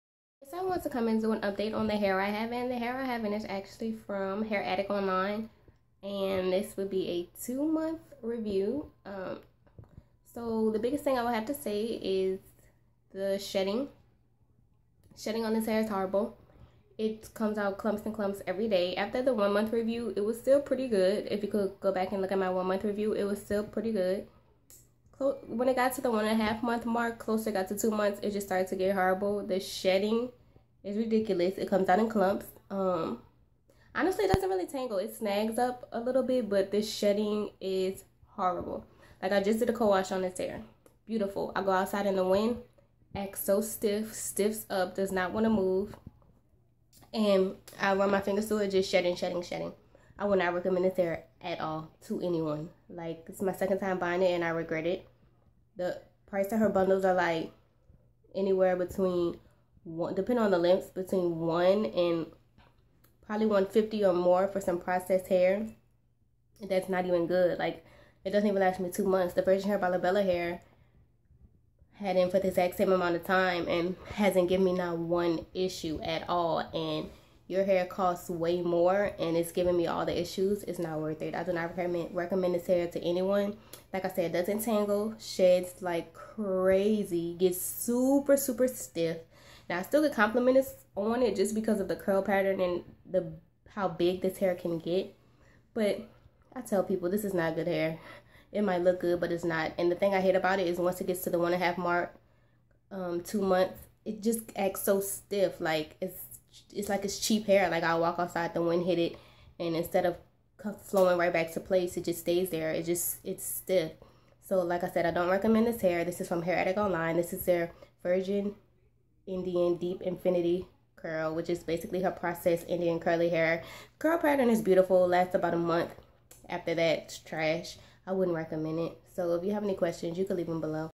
Hair Addict Online | 2 Month Final Review
☆Hair Installed:
Hair Addict Online- Virgin Indian Super Infinity Deep Curl

☆I DO NOT recommend this hair